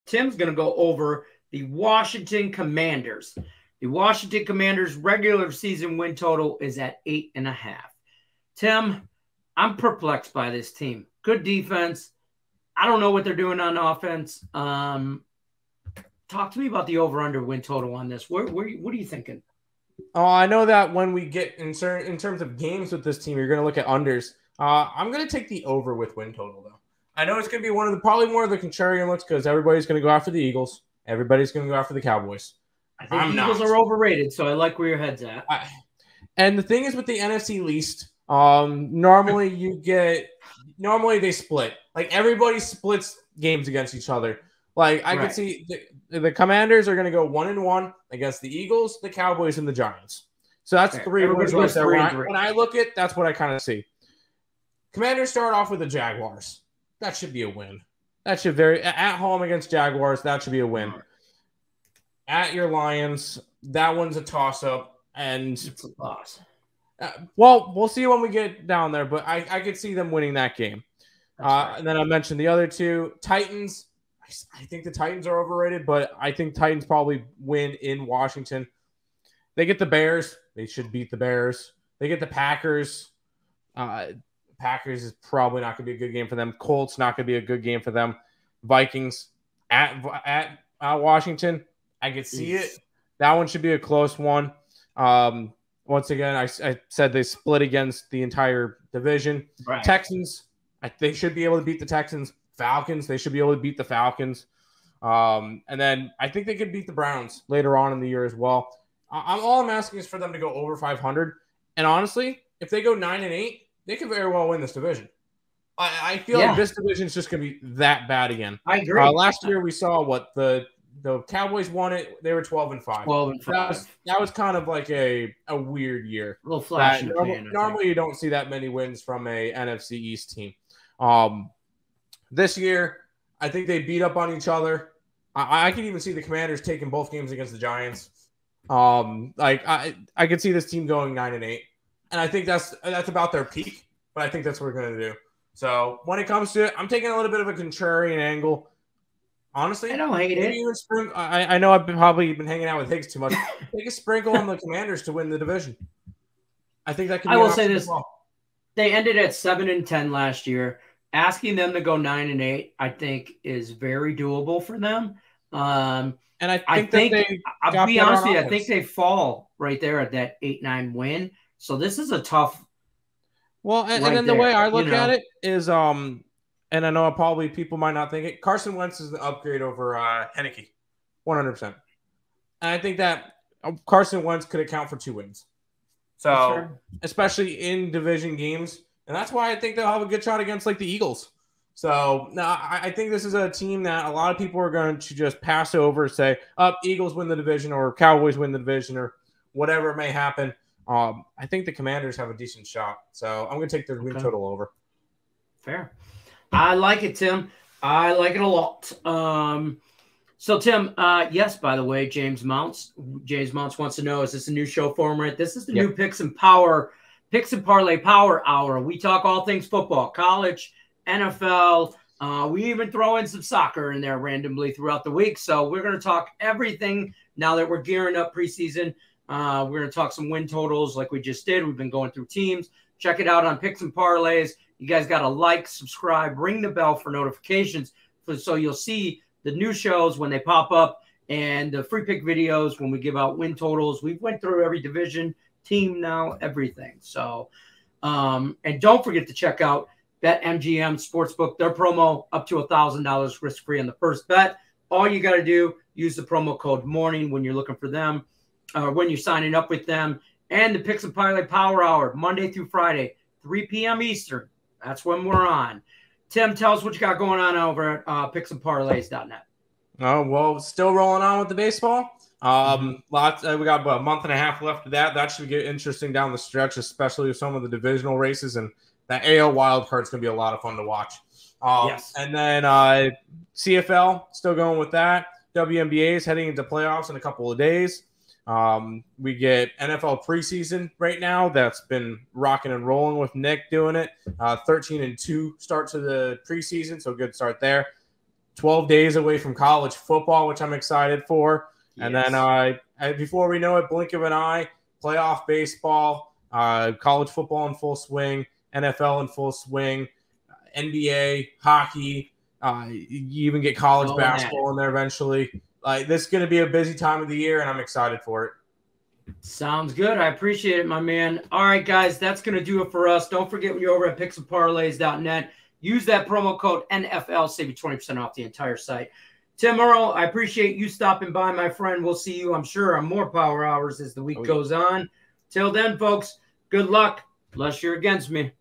Washington Commanders- 2022 Over / Under Win Total Picks & Prediction | Picks & Parlays
The NFL Season is just around the corner! Today we have a 2022 Season Preview for the Washington Commanders with expert Handicappers Detroit Lenny and Timothy Earle.